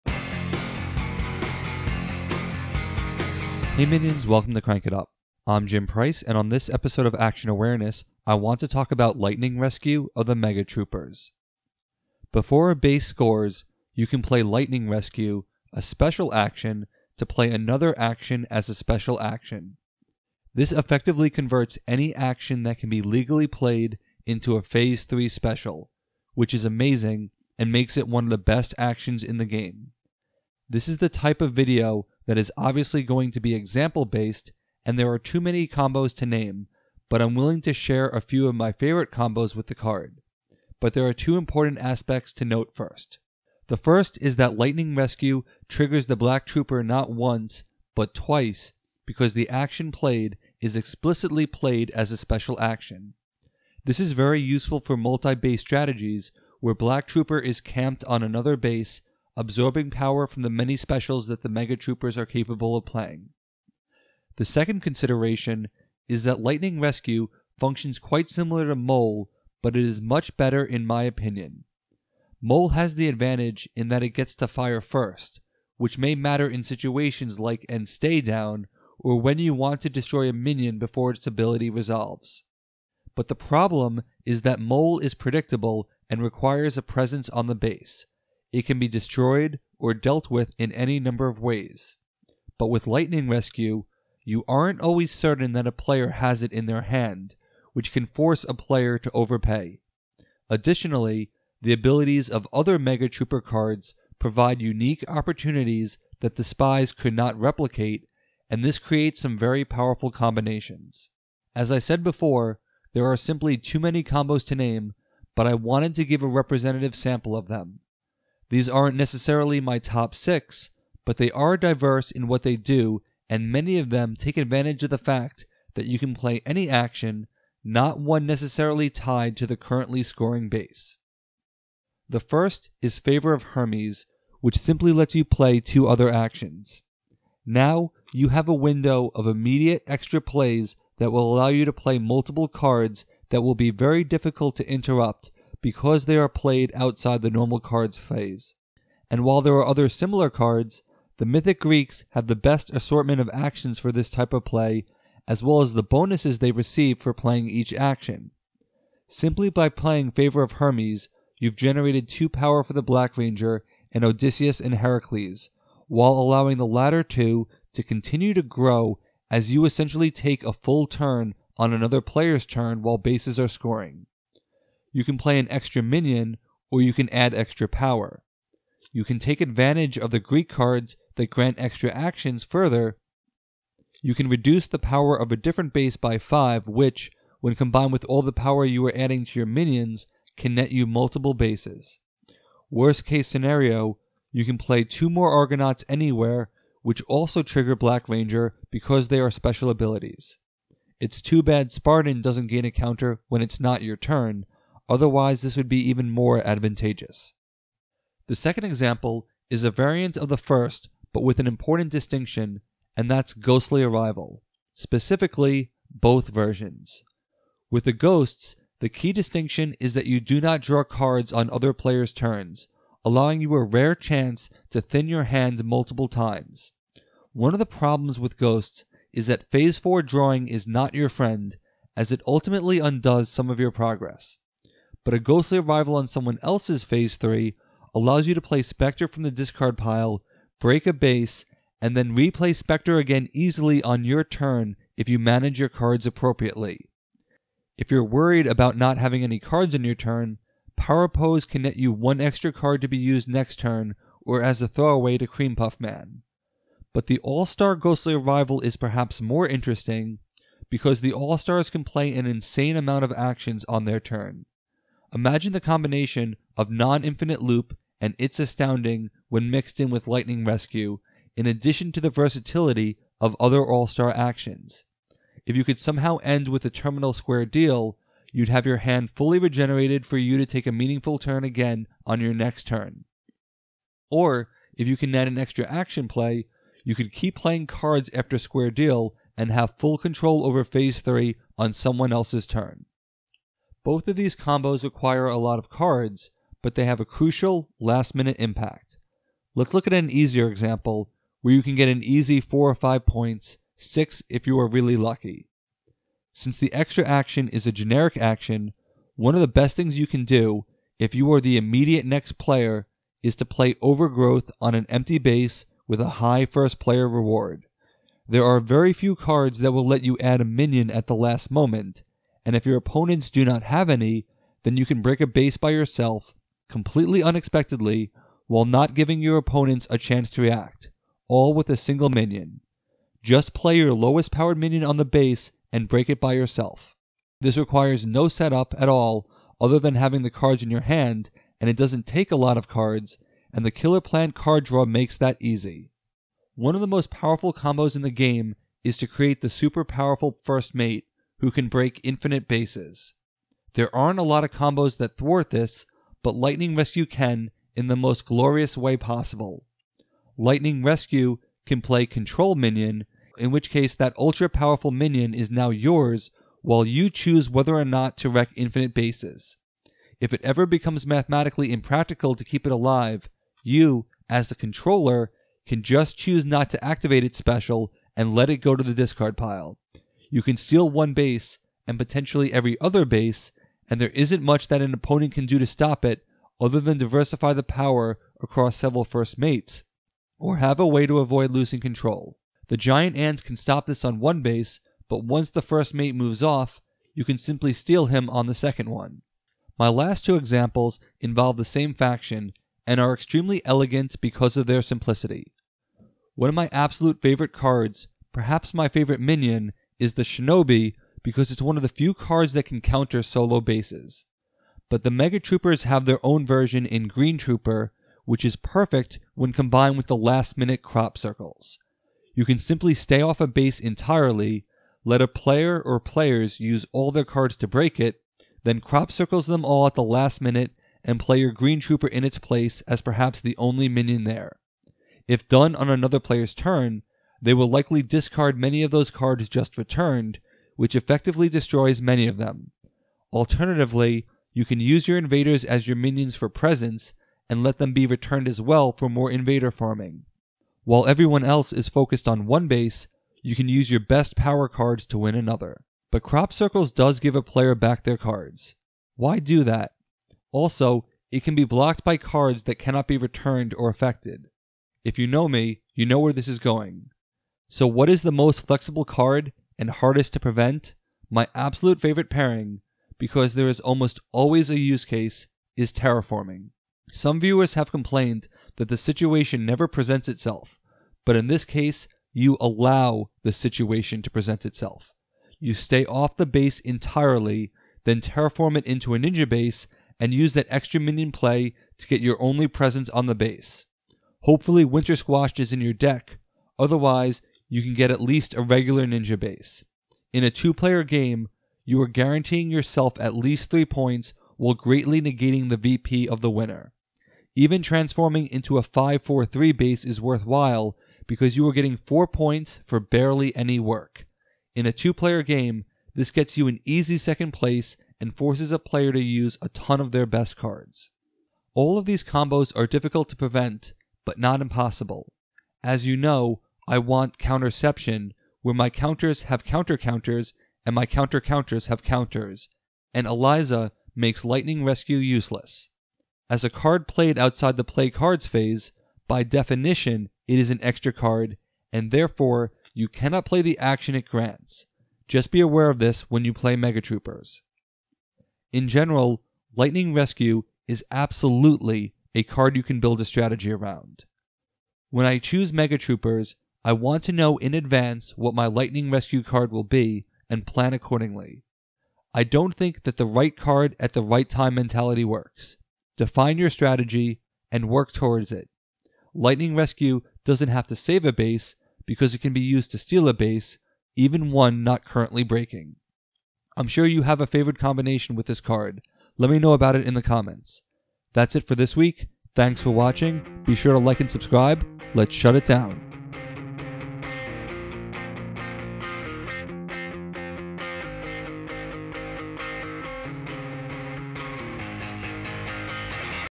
Smash Up Action Awareness #37: Lightning Rescue
Lightning Rescue can completely change the way Phase 3 unfolds and allow you to steal bases in many unexpected ways